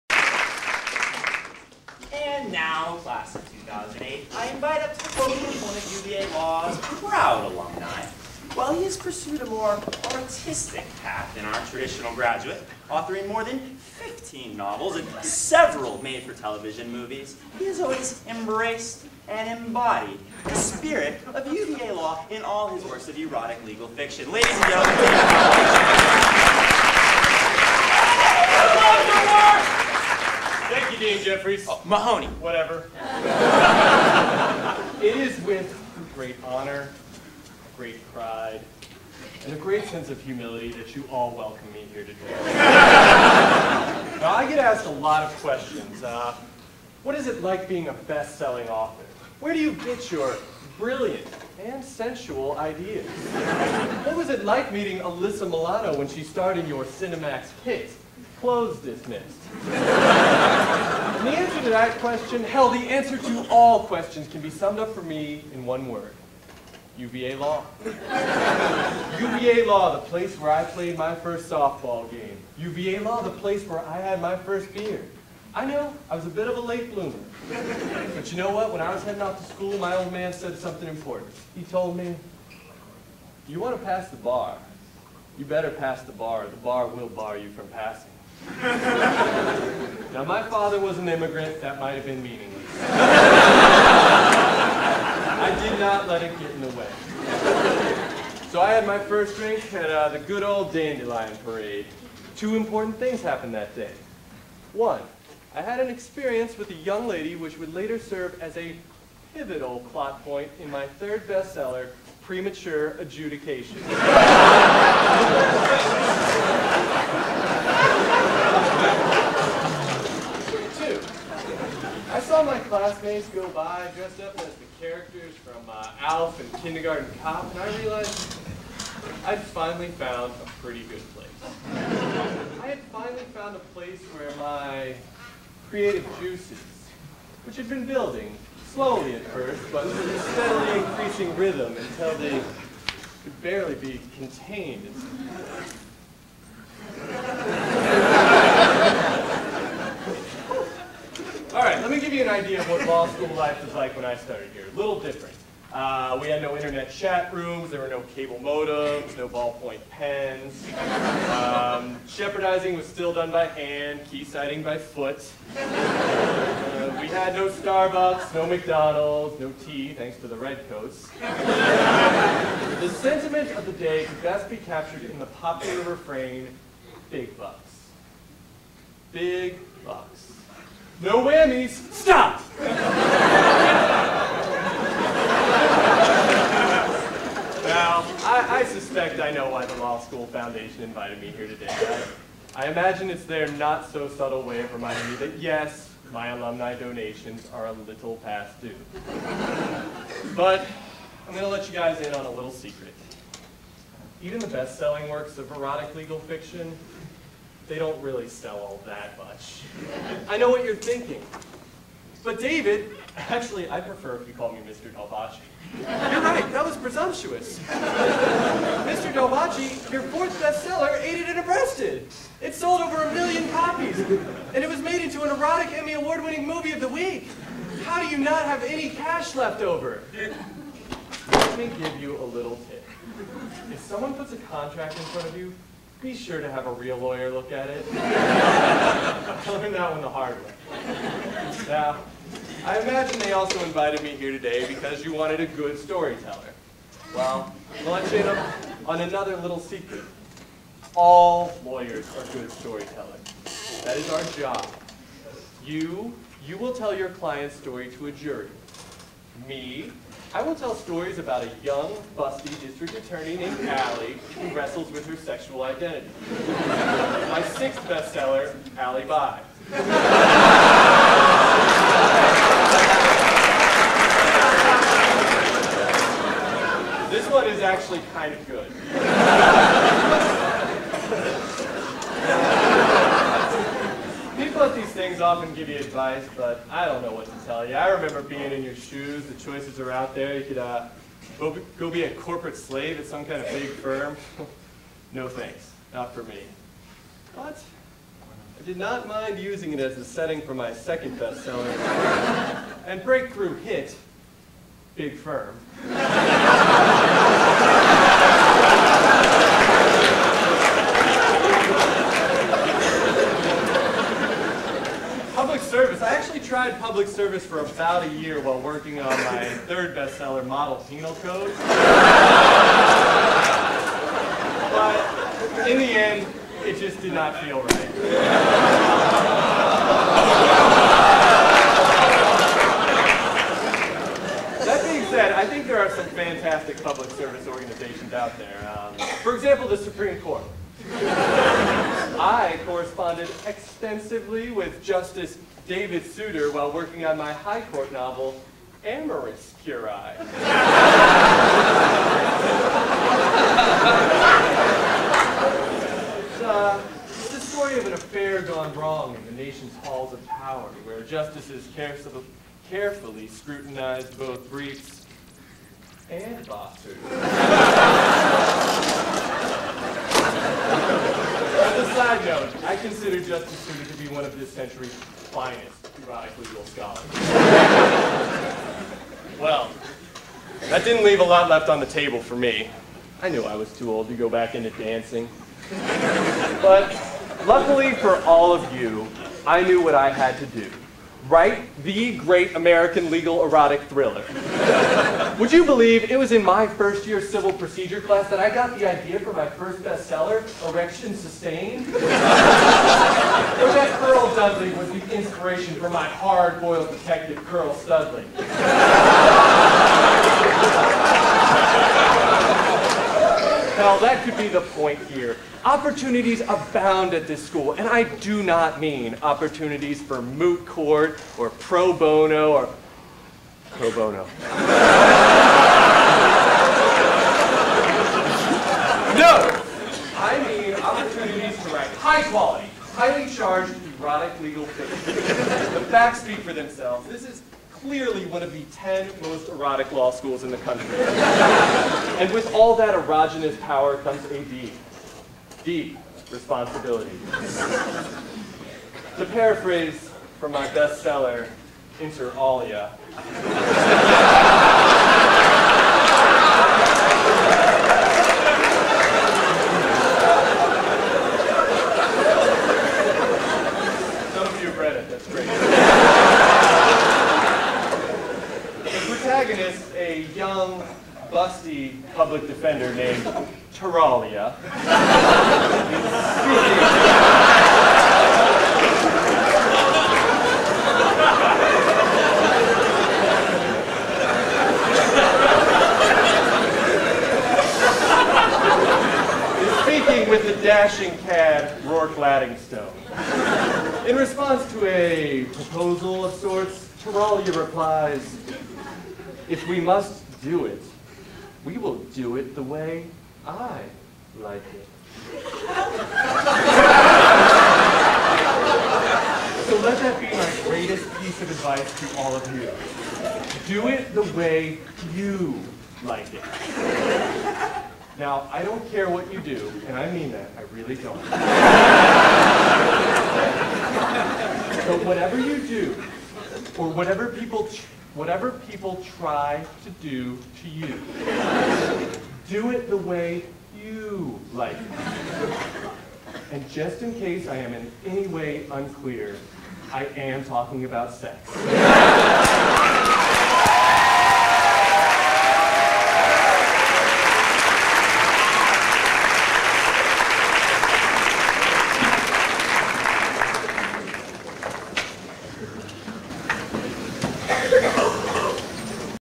David Baldacci Commencement Speech - Virginia Libel Show
A funny skit from the 100th Annual Libel Show at the University of Virginia School of Law.  Co-Director John Sheehan gives a tour de force performance as bestselling erotic legal fiction author David Dalbacci.  Not to be confused with legal author, UVA Law Grad and former commencement speaker David Baldacci.